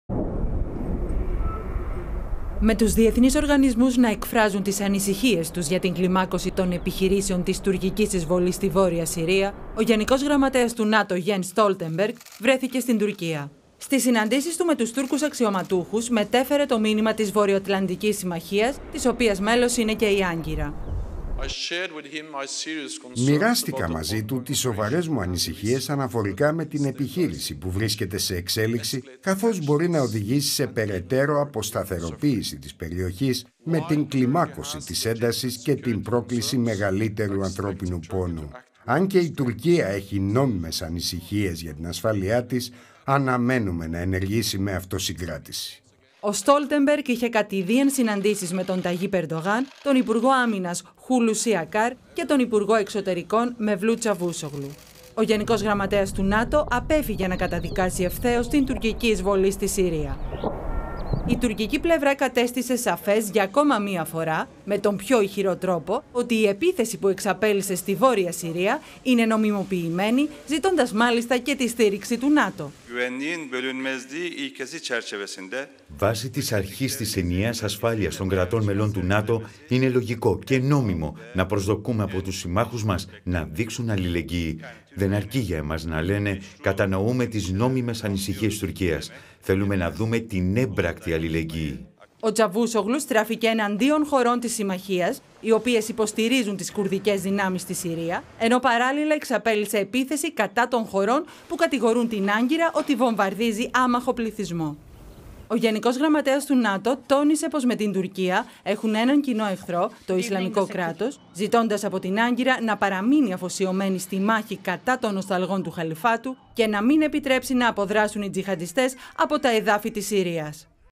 Στην Τουρκία ο ΓΓ του ΝΑΤΟ – Συναντήσεις με Ερντογάν και Τούρκους αξιωματικούς
Την υποστήριξη των συμμάχων της στο ΝΑΤΟ επεσήμανε ότι αναμένει η Τουρκία στην εν εξελίξει «νόμιμη» επιχείρησή της στη βορειοανατολική Συρία κατά την συνάντηση που είχε με τον Γ.Γ. του ΝΑΤΟ, Γένς Στολτενμπεργκ στην Άγκυρα, μια μέρα μετά την επίσκεψή του στην Αθήνα. Ο Στόλτενμπεργκ είχε κατιδίαν συναντήσεις με τον Ταγίπ Ερντογάν, τον Υπουργό Άμυνας, Χουλουσί Ακάρ και τον Υπουργό εξωτερικών, Μεβλούτ Τσαβούσογλου.Ο ίδιος σημείωσε ότι η Τουρκία θα πρέπει να δείξει αυτοσυγκράτηση, ενώ τόνισε πως η Τουρκία έχει νόμιμες ανησυχίες για την ασφάλειά της.